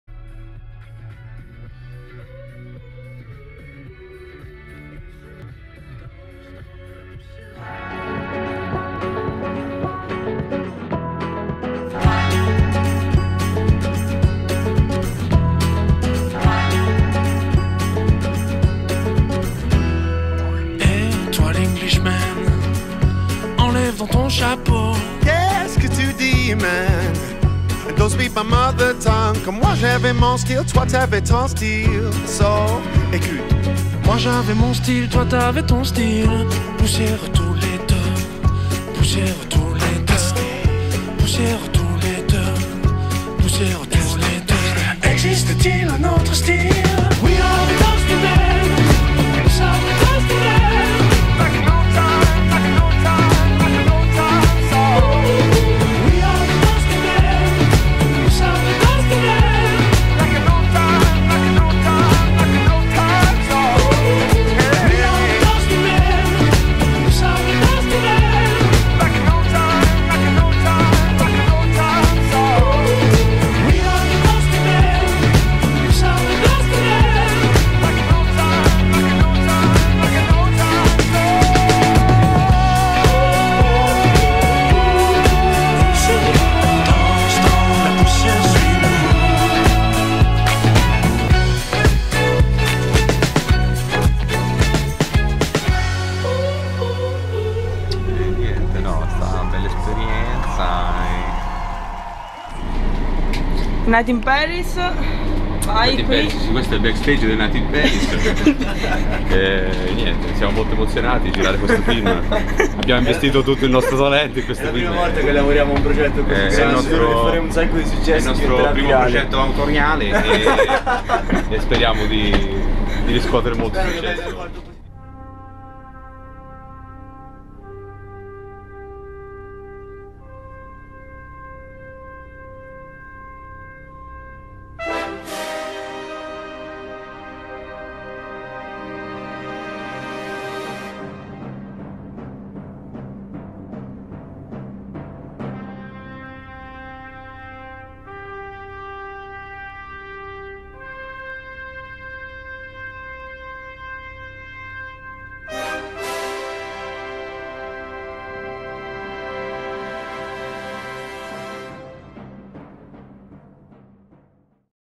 On va faire un tour à Angoulême
Festival de la Bande Dessinée d'Angoulême

 ma con pessimi risultati :)

Gopro 3 black edition e Fujifilm F750

Viaggio verso Angoulême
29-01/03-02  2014